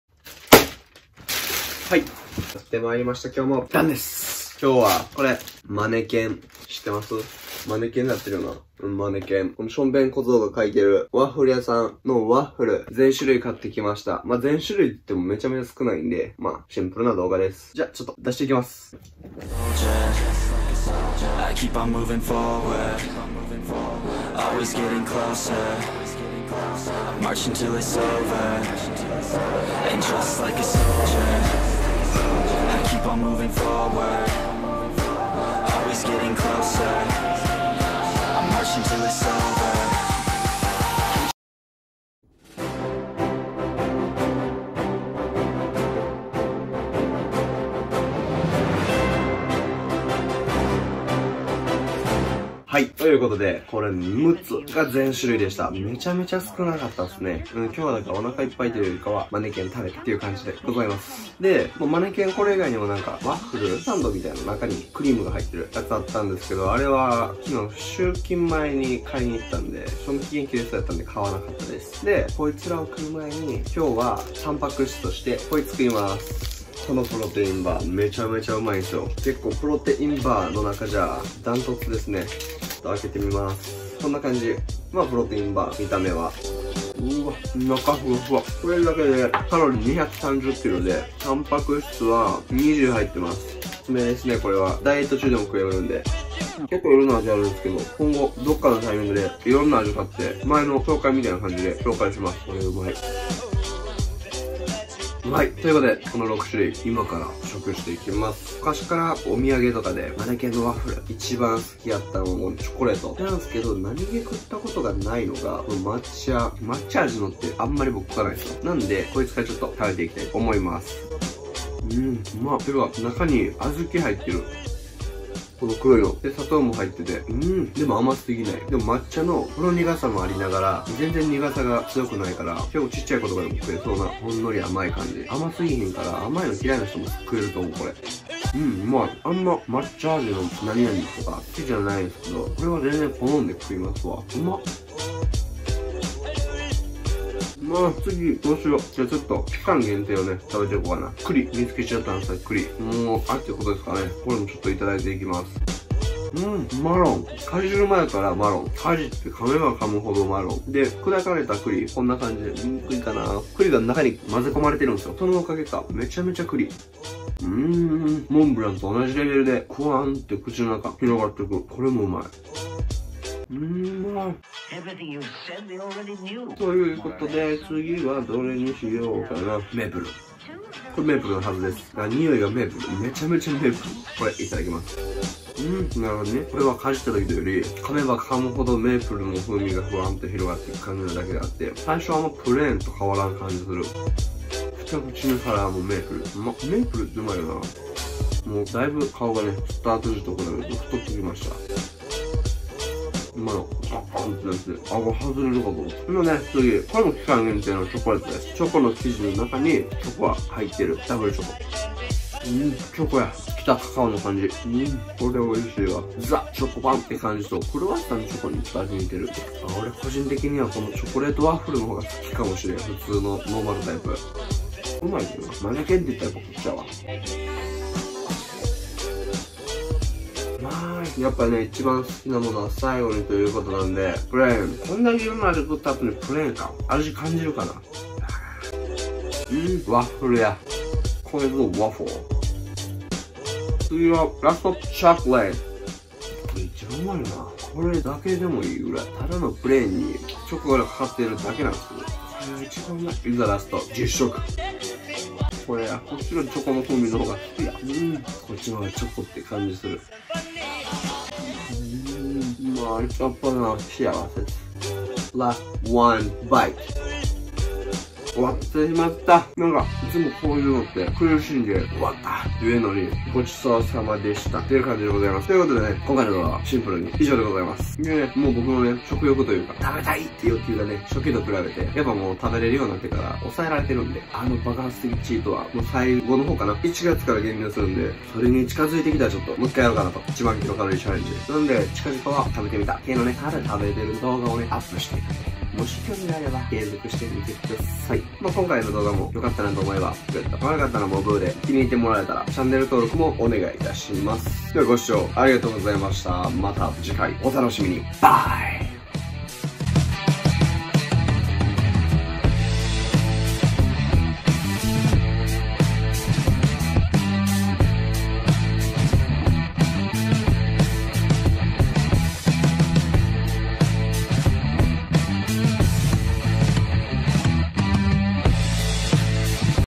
【マネケン】マネケン全種
マネケン全種全部美味い！！
ワッフルと言えばマネケン！！